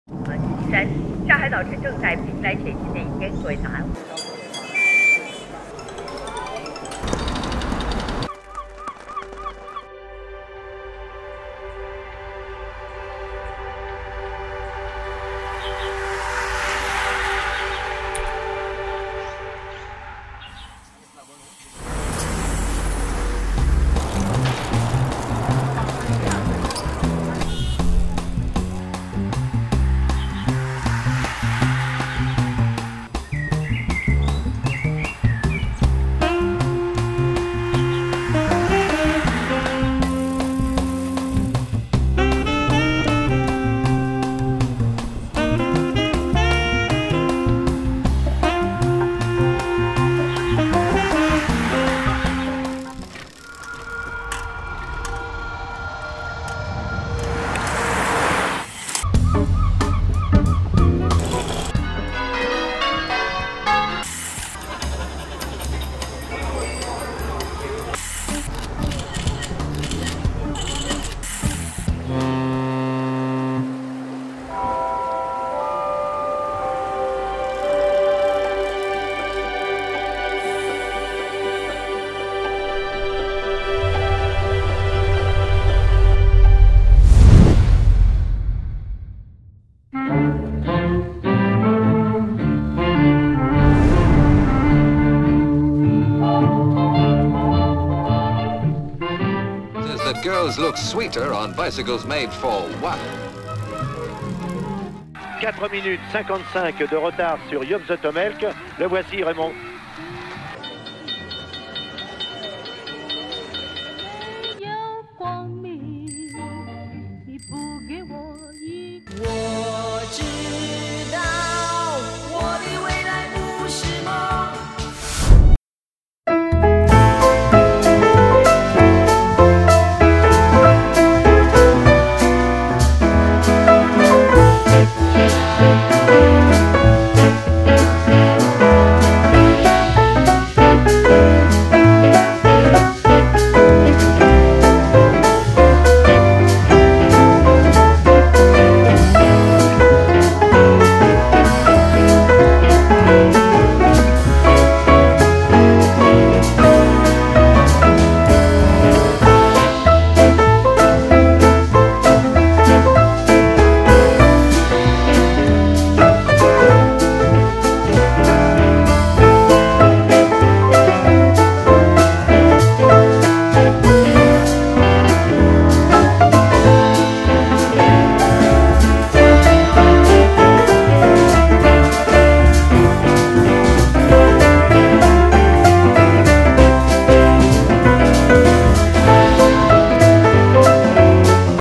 Discover Shanghai on a bike
A mini documentary about cycling culture in Shanghai